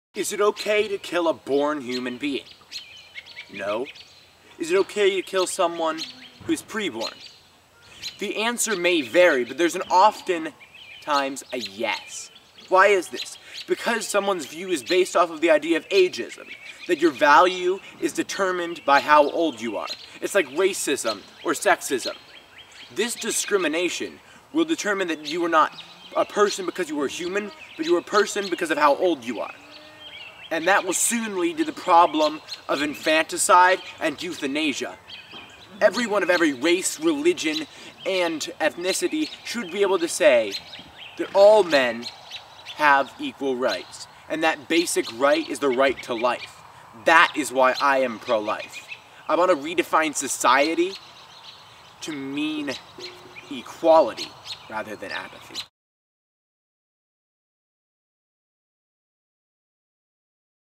Ohio Right To Life Video Contest (Ageism: Abortion, Euthansia, and Infanticide)
My submission of Ohio Right to Life's "Why am I Pro-life" video contest (ohiolife.org).
Oh and the video is ACTUALLY 60 seconds, but it shows up (like everyone else's video) as one second over. I'm not sure why.